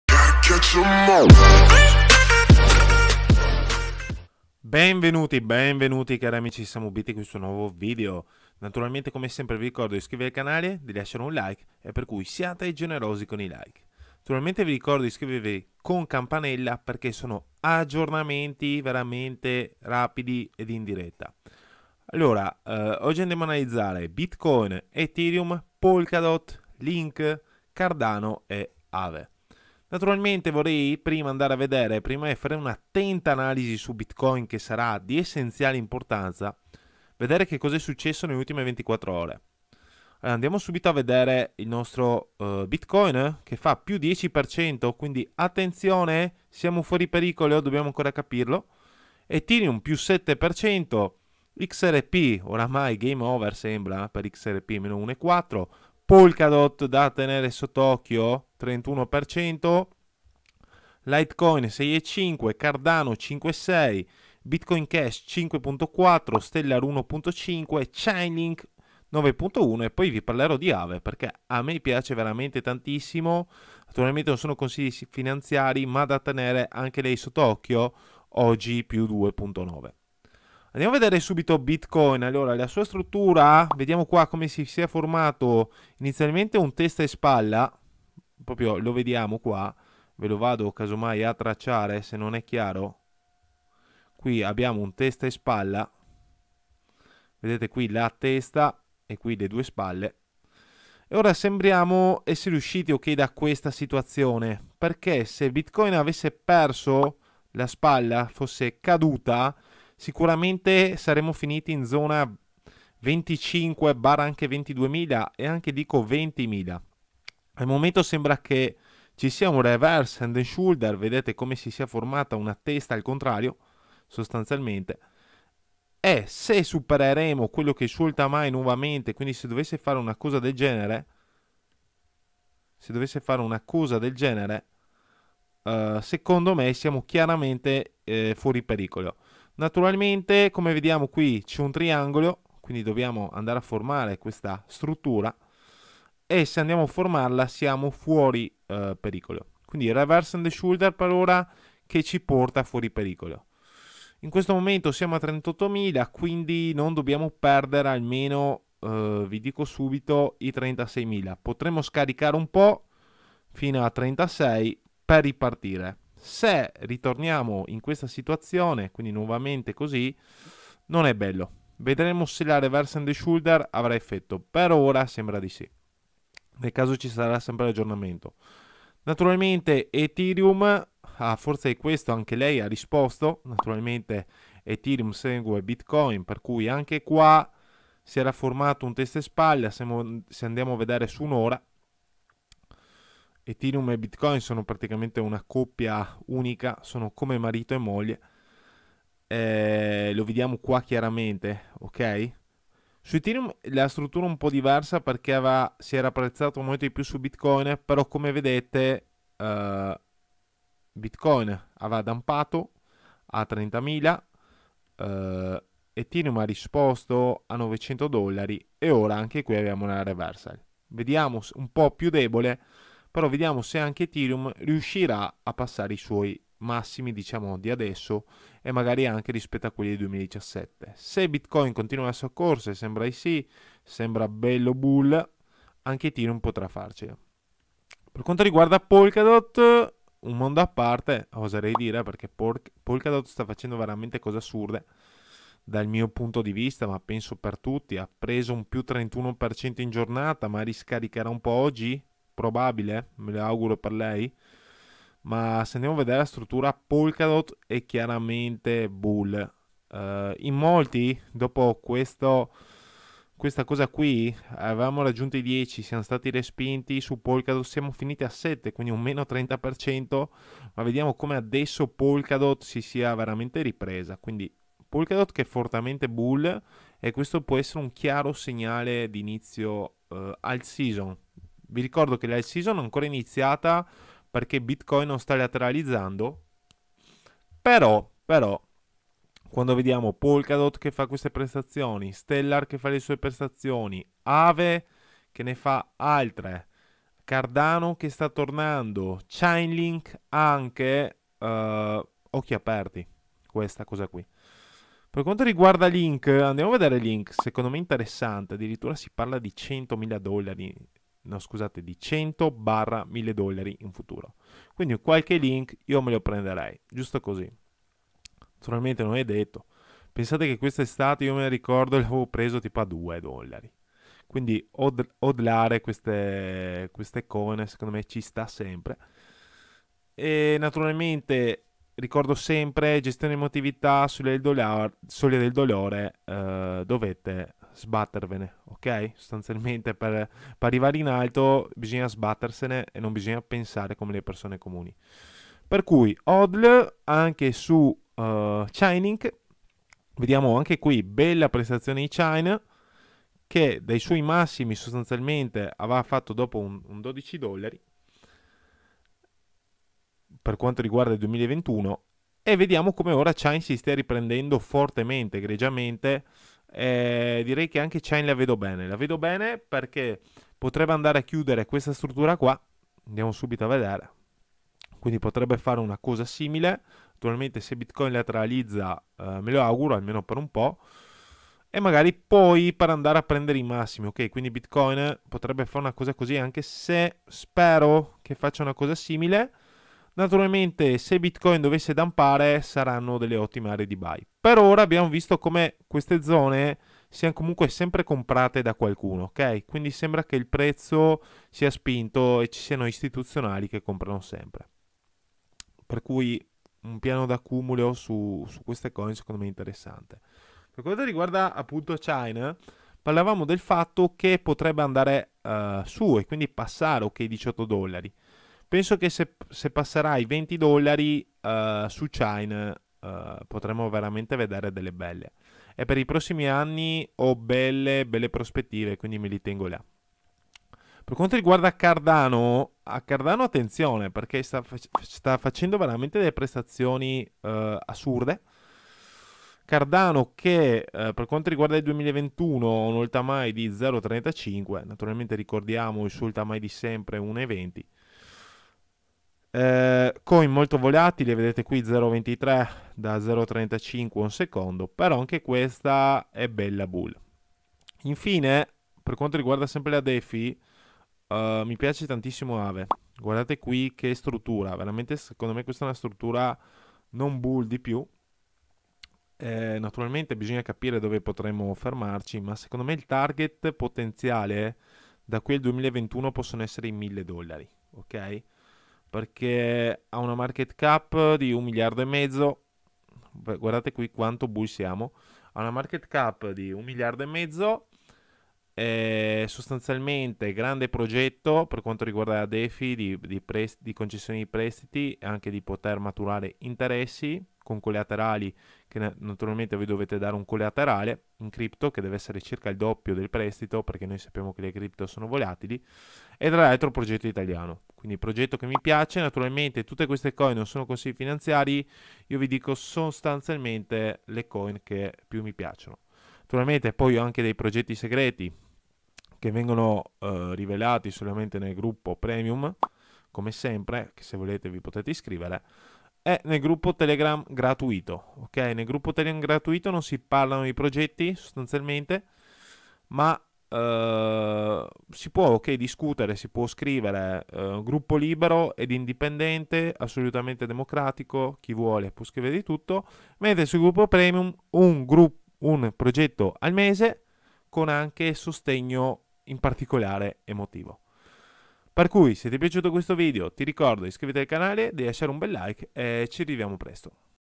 🔥 BITCOIN ED ETHEREUM SIAMO FUORI PERICOLO? DOT A BOMBA, ATTENZIONE AD AAVE 🔥
Benvenuti su Samu Bit,

Imparerete dove comprare, come custodire le vostre valute e le ultime novità e sviluppi sui progetti più interessanti.

Bitcoin ed Ethereum siamo fuori pericolo? Per il momento sembra il trend sia in discesa sia nuovamente in risalita, ma attenzione.

Polkadot stà sorprendendo ma attenzione pure ad Aave entrambe in zona discovery. Link tiene bene e presto potrebbe risvegliarsi.

L'uso della leva finanziaria è estremamente rischioso. NON utilizzarla se non ne comprendi il funzionamento e i rischi.